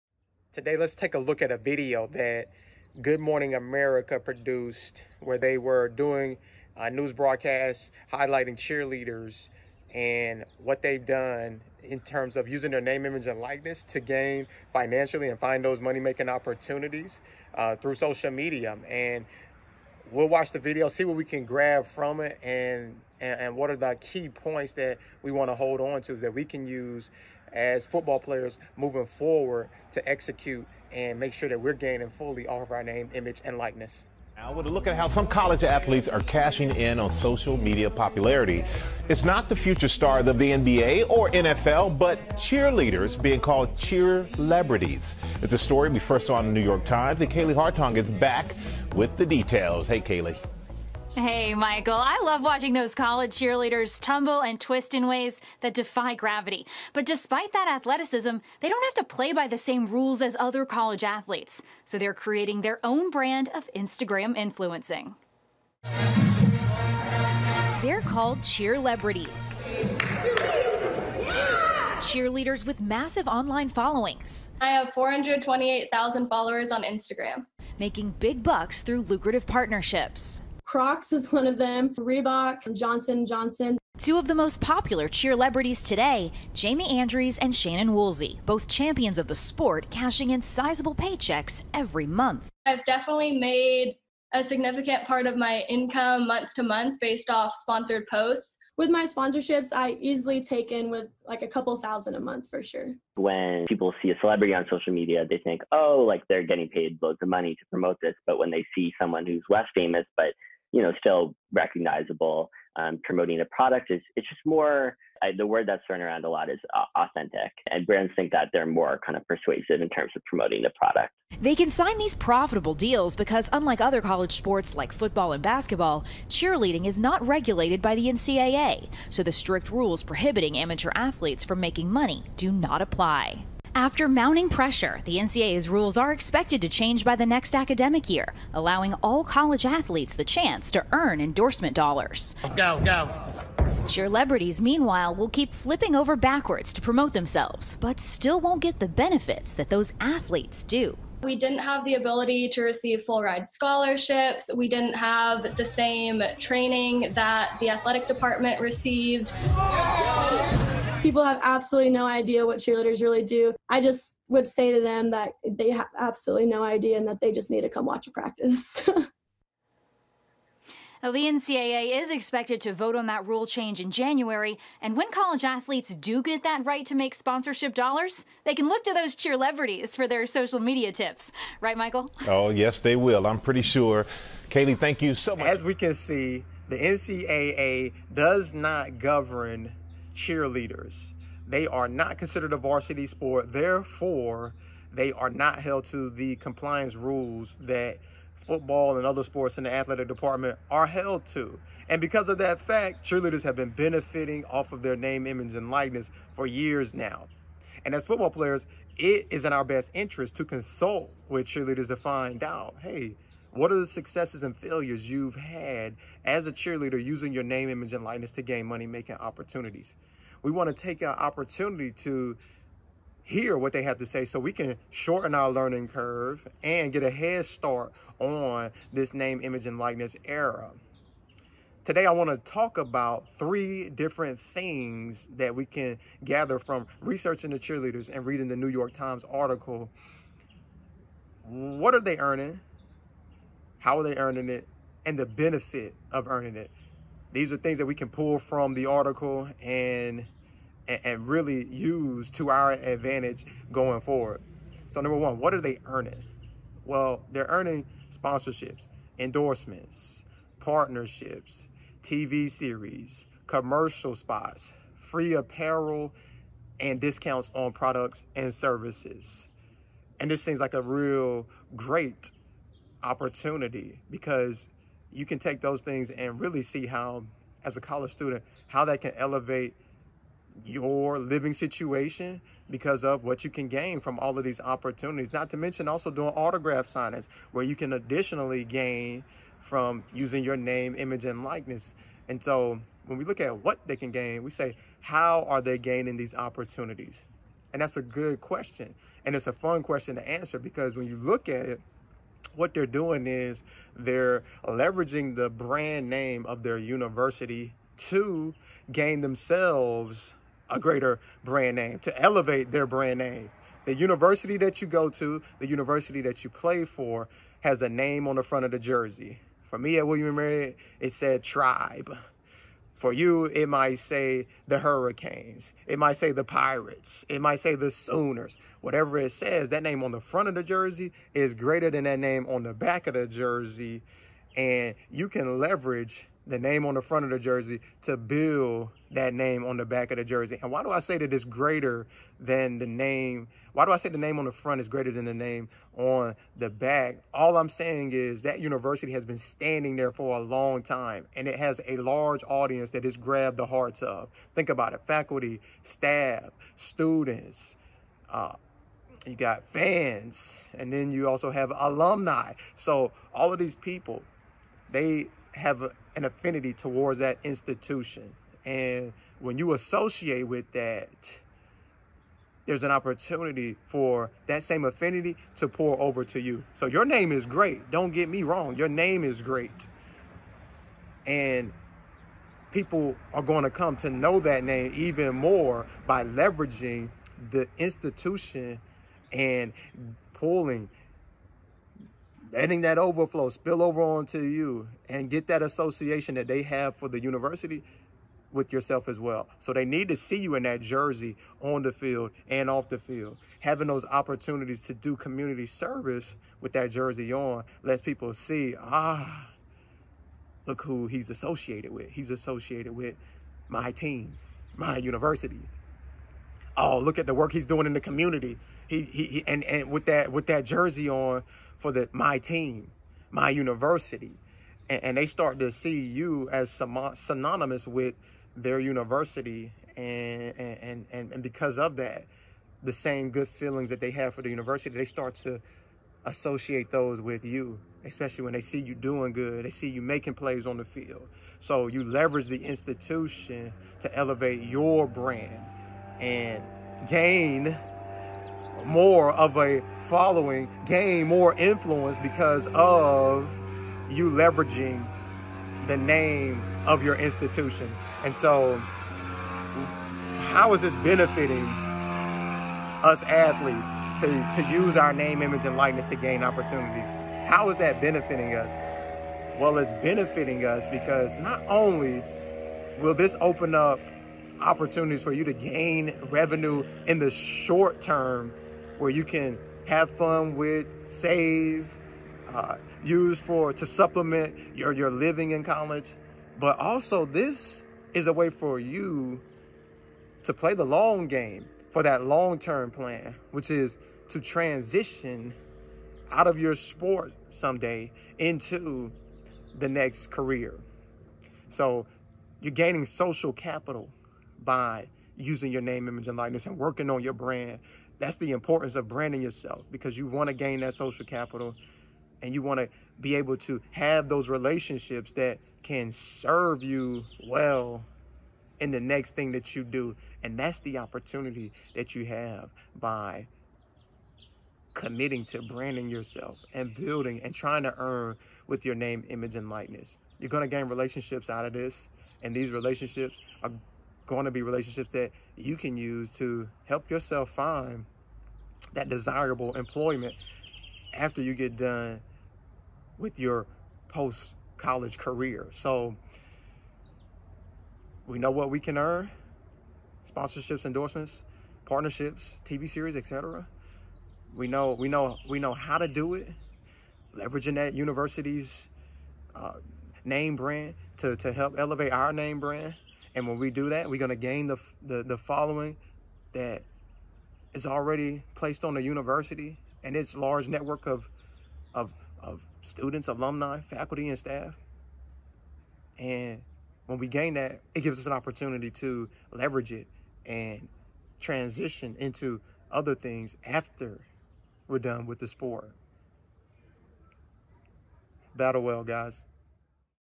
CheerLEADERS in NIL
College football players, if you want to start earning off of your name, image, and likeness (NIL), take notes from collegiate cheerleaders. They are not governed by the National Collegiate Athletic Association (NCAA). Therefore, cheerleaders have been earning endorsement dollars for years off of their NIL.

Leverage their experience to gain a perspective on how to fully benefit from branding yourselves with your NIL.